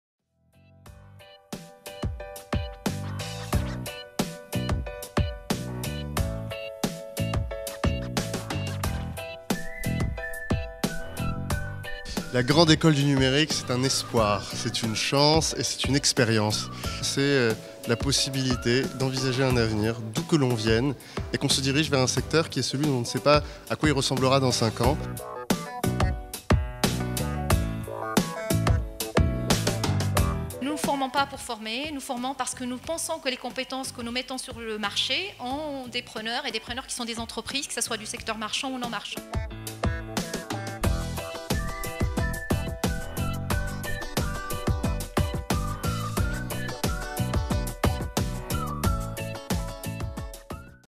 Soirée de présentation du rapport d'activité 2016
Le mardi 30 mai 2017 à l'Espace Batignolles à Paris, la Grande Ecole du Numérique a dévoilé son premier rapport d’activité en présence de Mounir Mahjoubi, secrétaire d'État chargé du Numérique !